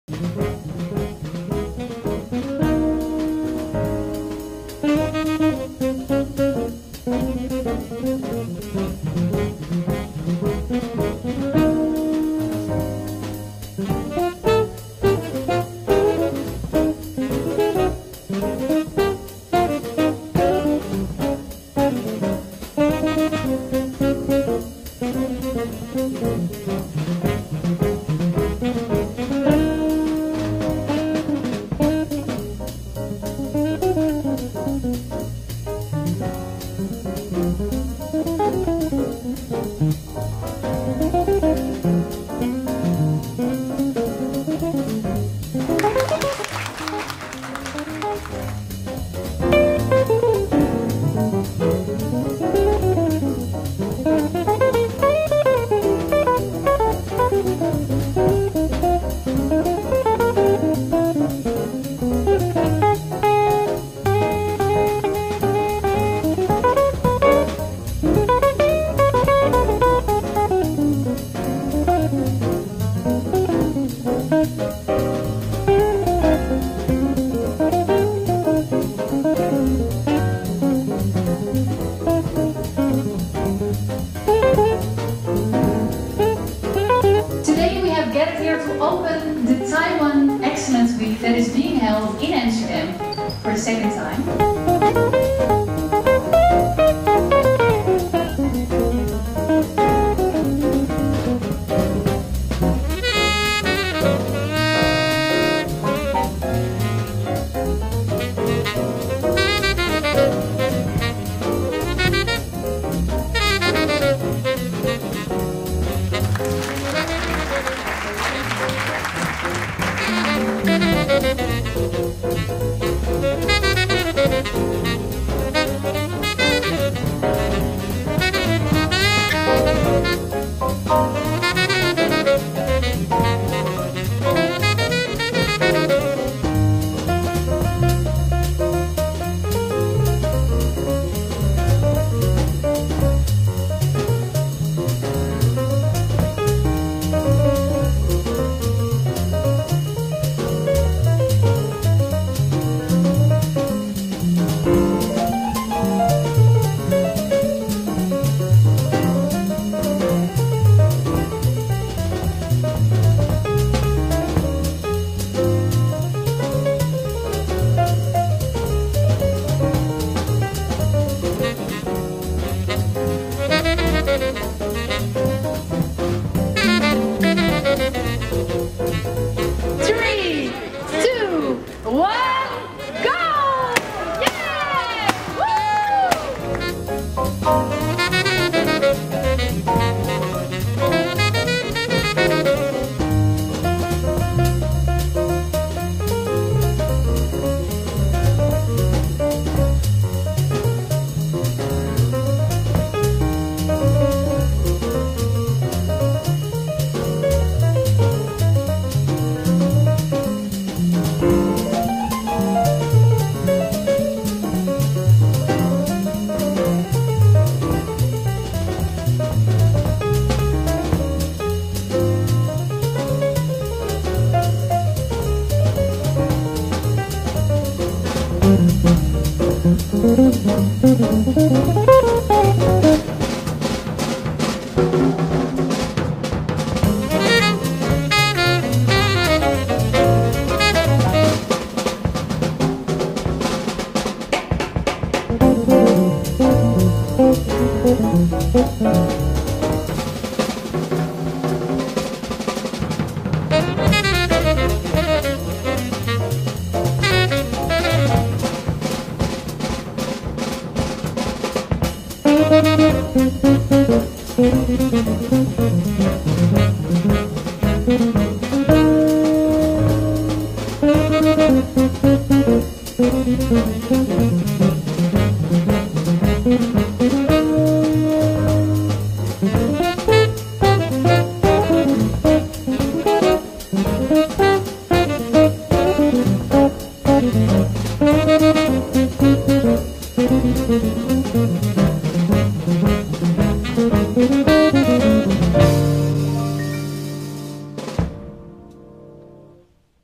2015年荷蘭阿姆斯特丹臺灣優良產品體驗活動
2015年6月23至28日外貿協會(TAITRA)帶領廠商於荷蘭阿姆斯特丹辦理台灣精品體驗店，展出53家品牌93件產品，6月23日開幕由葉副秘書長明水主持，現場冠蓋雲集，逾百餘位貴賓及20餘位媒體出席，貴賓包括我國駐荷蘭代表處周台竹大使、荷蘭貿易推廣協會主席Dr. Jan Siemons、阿姆斯特丹中小企業協會主席Bart Drenth，台商會會長黃榮光、荷蘭商業總會(KVK)代表Jacob van der Vis及各家台灣精品廠商代表等。媒體則包括荷蘭主流媒體RTL7電視台，最大通訊社ANP，華人廣播及電視台CRTV及第一大報Telegraaf派員採訪，此外亦吸引專業媒體如Fitness News、Hardware Info及Techlife專訪出席的台灣精品廠商。6天活動吸引約5萬人參觀，媒體報導觸達人數約200萬人。